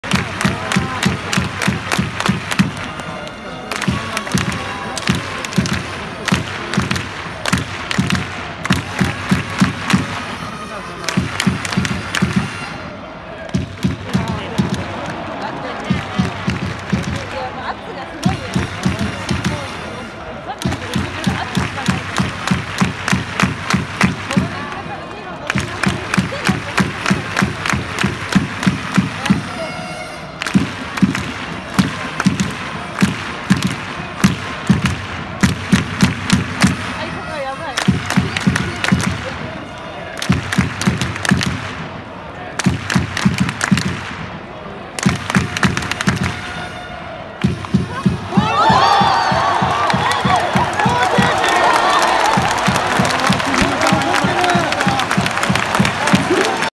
宮本選手 意地の内野安打
2020/11/14
JERAセ・リーグ公式戦 24回戦
横浜DeNAベイスターズ
VS

今シーズン最終戦